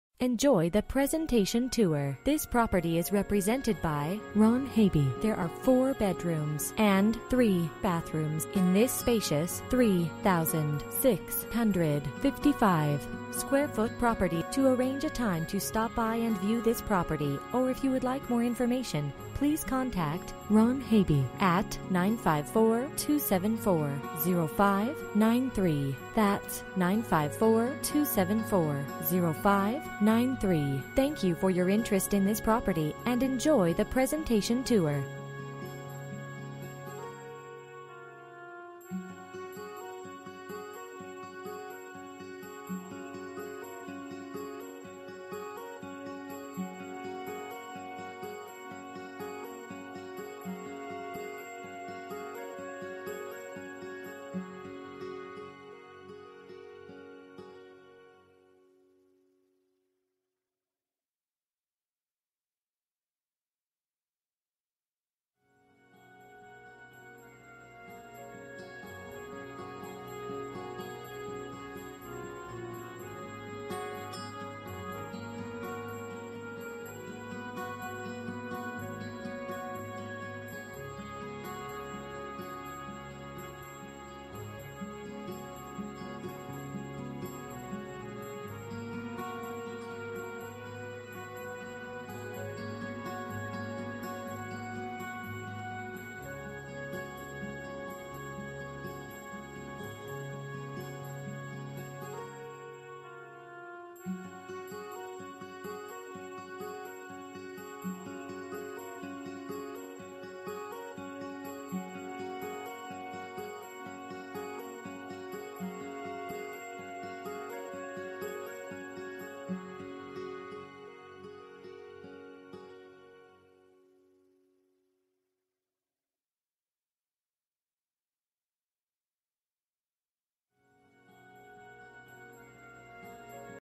for sale - 2585 Jardin Court, Weston, FL
Awesome Waterfront Pool Home on Oversize lot in the Prestigious Weston Hills Country Club. Gorgeous curb appeal, lush landscaping, circular driveway with brick pavers. Amazing water views from Living Room, Kitchen, Family Room and Master Bedroom. Open floor plan w/ high ceilings, crown molding, lots of windows and natural light. Nice layout, huge Master Bedroom w/ sitting area, double closets and huge Master Bathroom, built in Home Audio System with 2 speakers in each room, open kitchen w/ custom cabinets, serving bar w/ work station & sink. Enjoy entertaining guest in your shiny bright pool and Jacuzzi overlooking the lake, south exposure. Home is well insulated with R30 Insulation and features accordion hurricane shutters. Great schools, parks, dining, shopping and everything you can think of.

Bedrooms: 4
Bathrooms: 3 bathrooms
Square Feet: 3655
Price: $729,000

MLS ID: F1269914